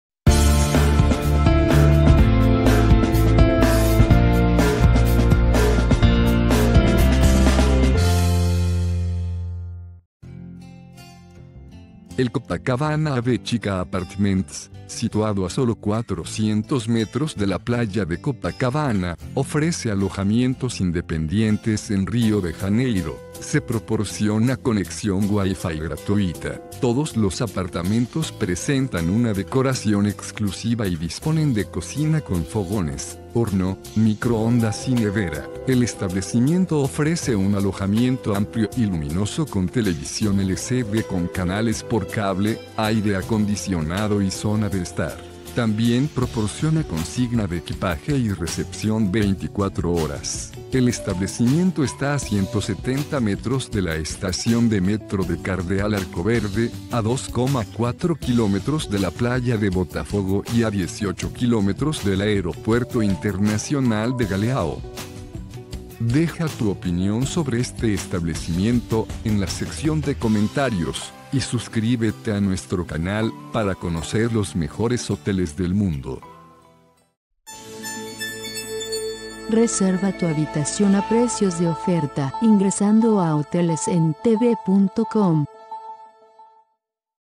Copacabana AV Apartments, Apartamento en Rio de Janeiro
Copacabana AV Apartments incluye recepción 24 horas, habitaciones no fumadores y habitaciones familiares. El edificio se encuentra ubicado en Rua Barata Ribeiro, 181 y los pasajeros de Copacabana AV Apartments le asignaron una calificación de 7.9. Quienes quieran alojarse en la ciudad de Rio de Janeiro encontrarán en Copacabana AV Apartments una buena propuesta.